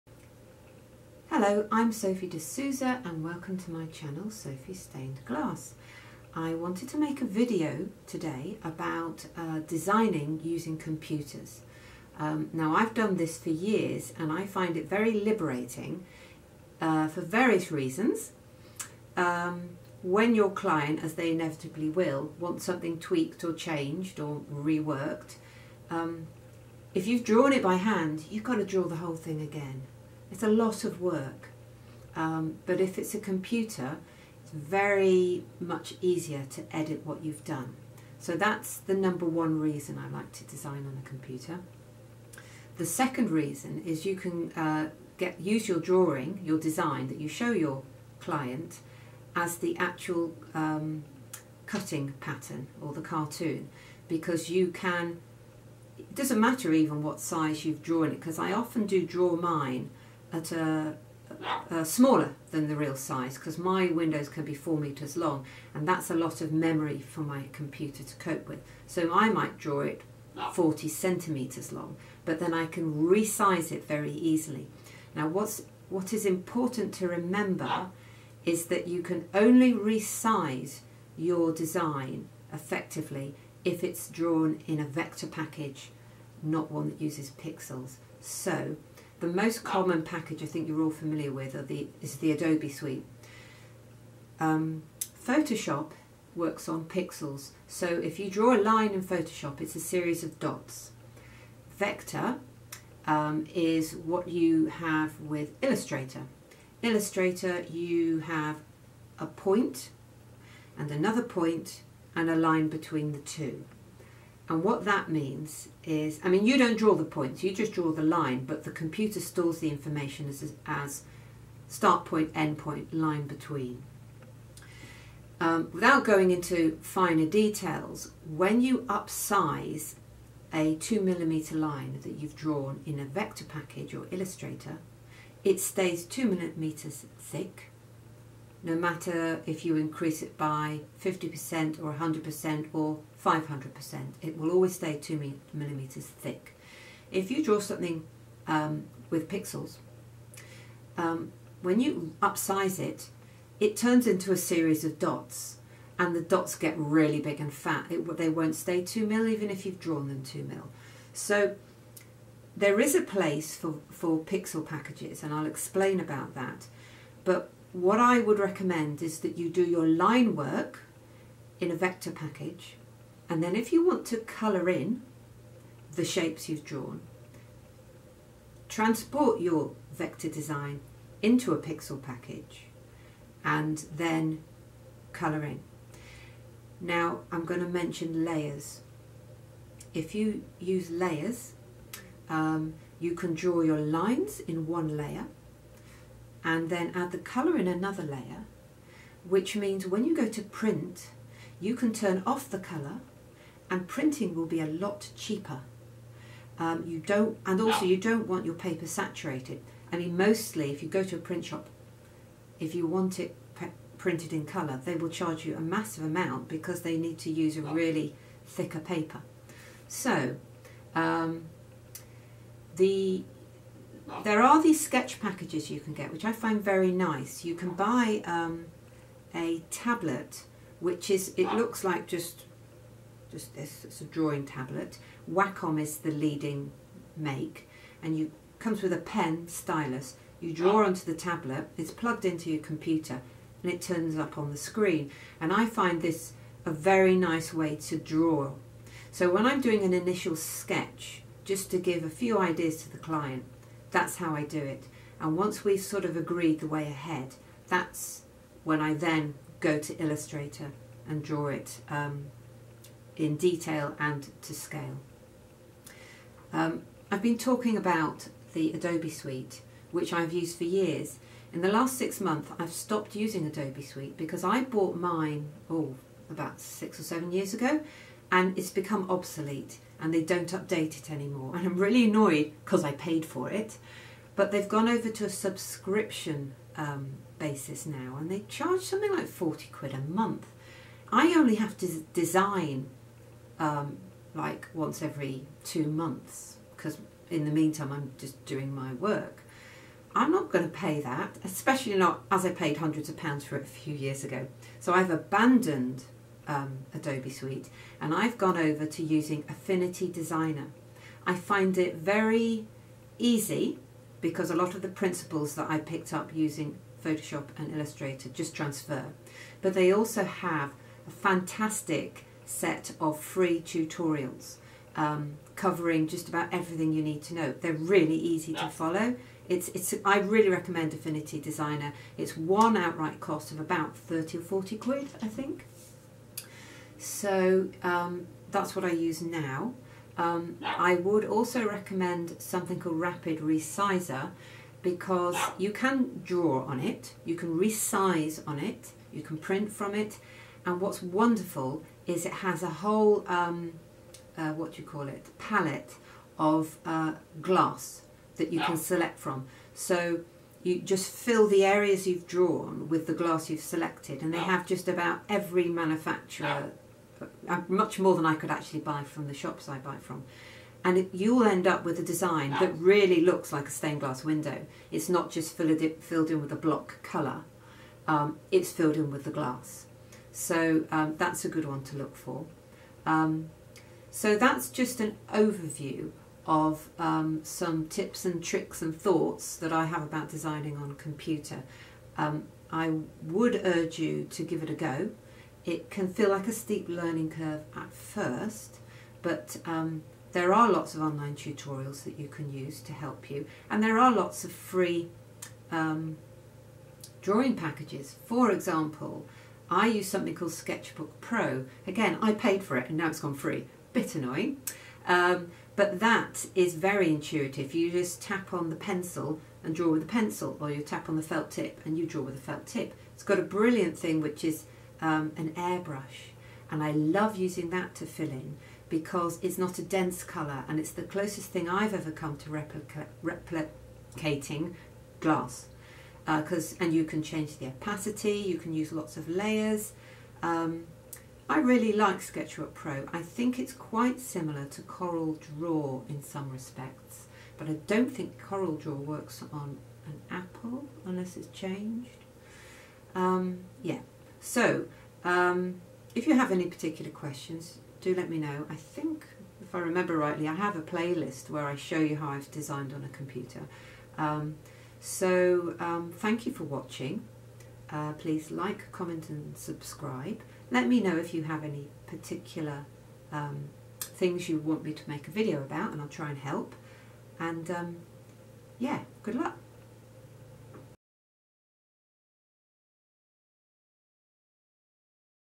Sophie's Stained Glass | Designing Stained Glass on a Computer
A brief overview of the options for producing your designs on a computer. More videos on this subject can be found in the 'Designing' playlist.

Find out more about Sophie's domestic and ecclesiastical stained glass work by visiting